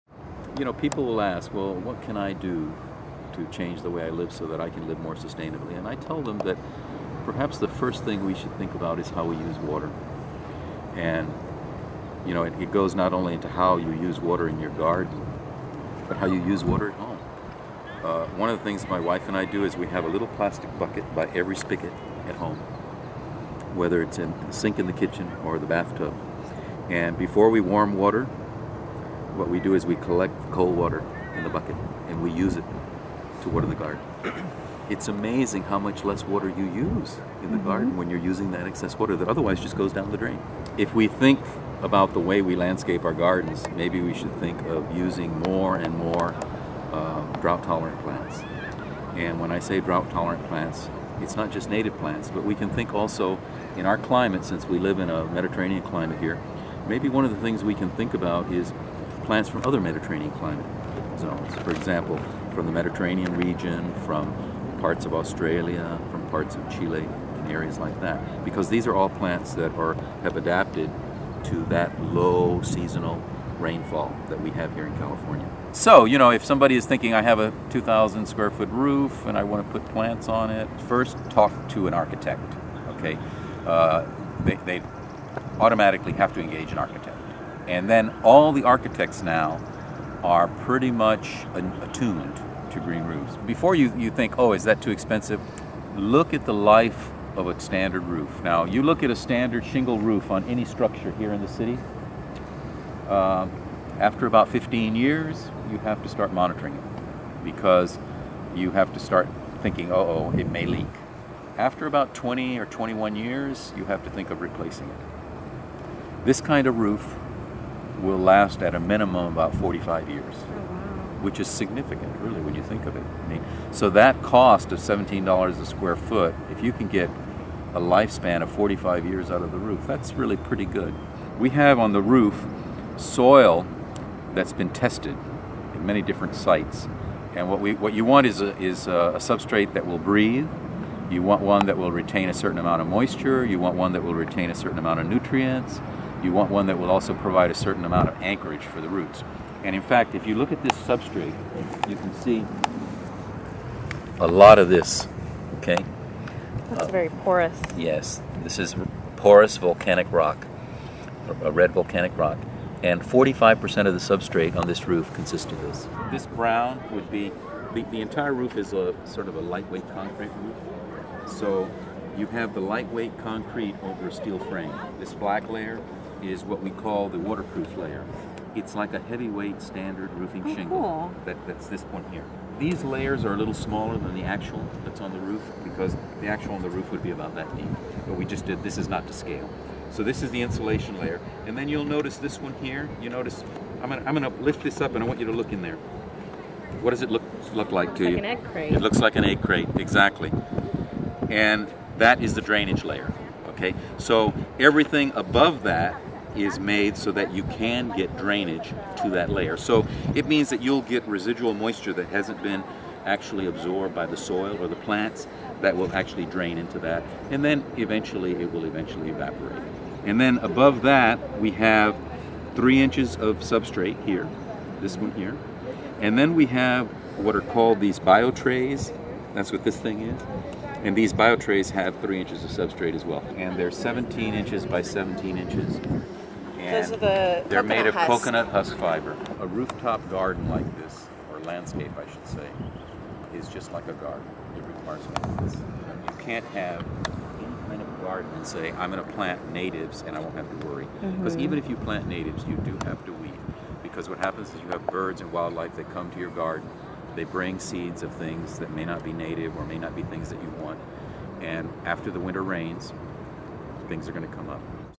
Asterisk San Francisco Magazine - California Academy of Sciences Living Roof with Dr. Frank Almeda
Asterisk San Francisco Magazine Video
Watch a tour of the California Academy of Science's living roof with Dr. Frank Almeda. Discussing the layers that makes the roof, sustainable living and how to adopt simple green initiatives to save mother nature.